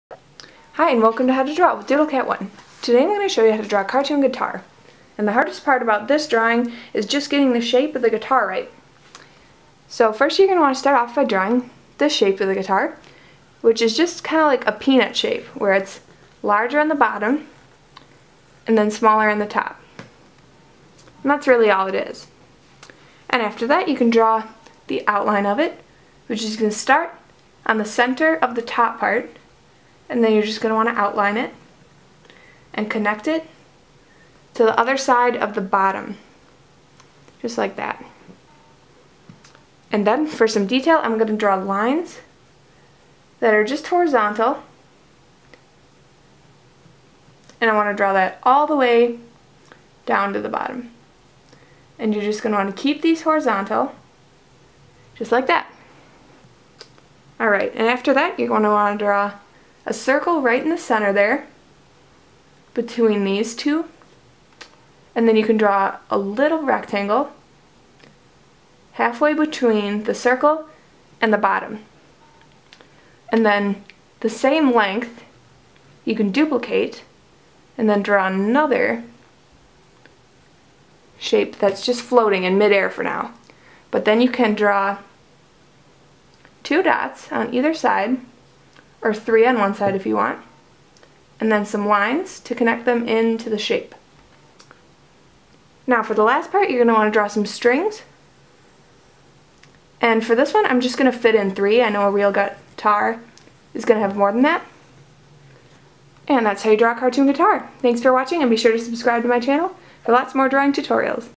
Pk Pk How to Draw a Cartoon Guitar
Pk Pk How to draw a simple cartoon guitar in less than 2 minutes! Difficulty level 3/5.